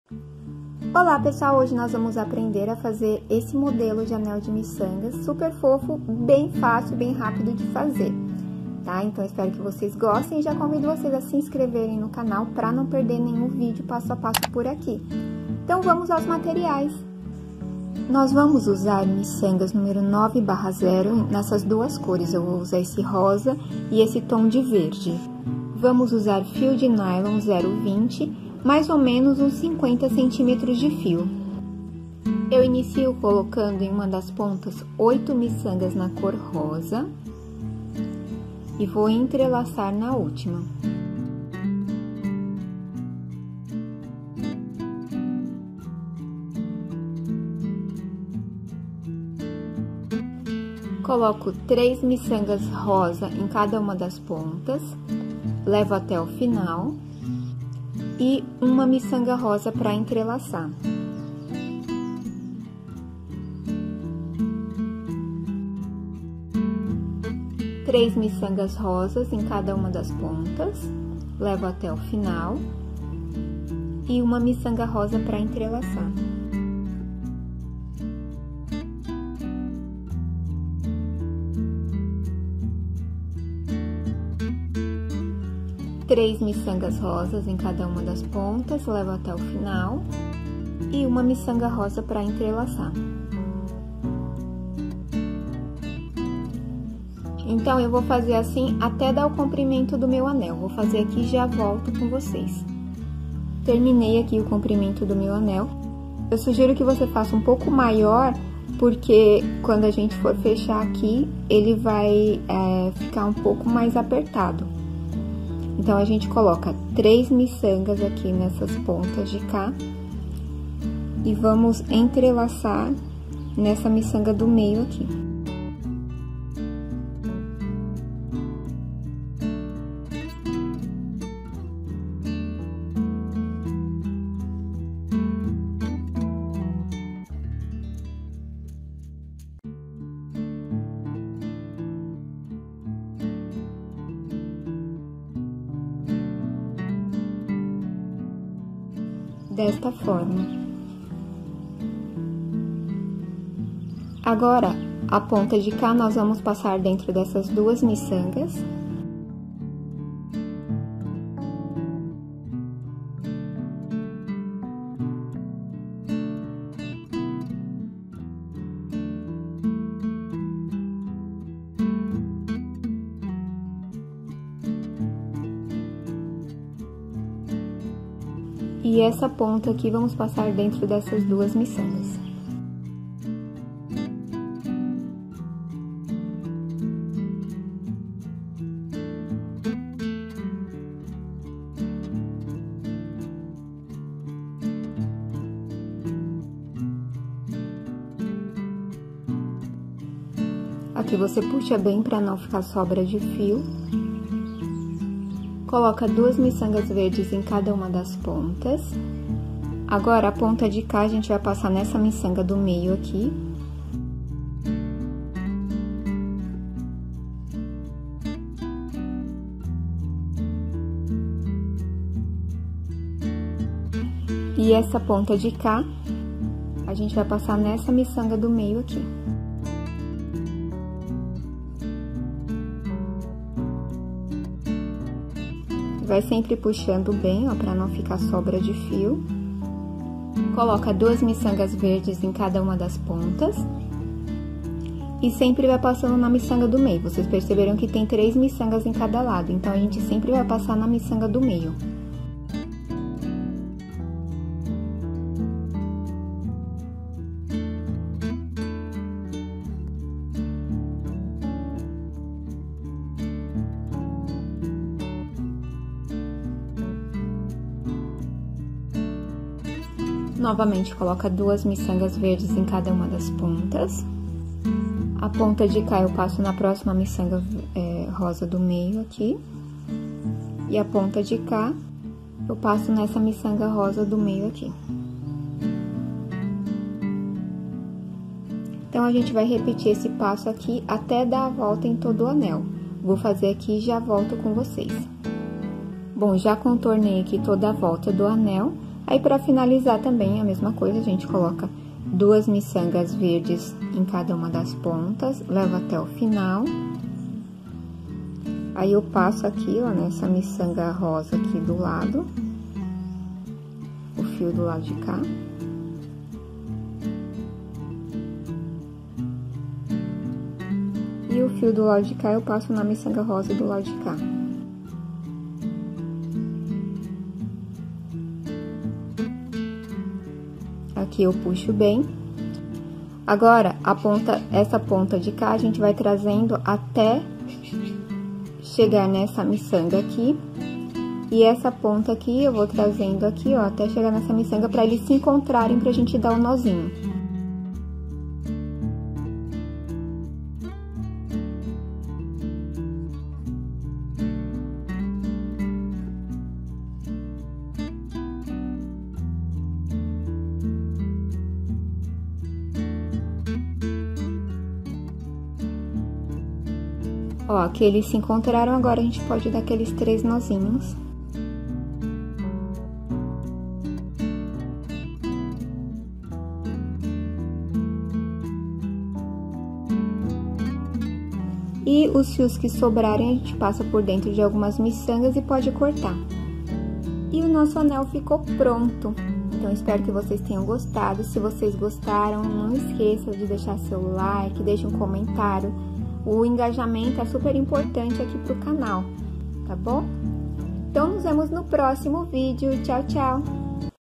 Como fazer anel de miçangas fácil e rápido passo a passo | Beaded ring tutorial
Neste vídeo ensino a fazer este lindo anel de miçangas. É um passo a passo bem fácil e fica lindo.
Os acessórios de miçangas estão super em alta, aproveite esta tendência e faça vários modelos lindos!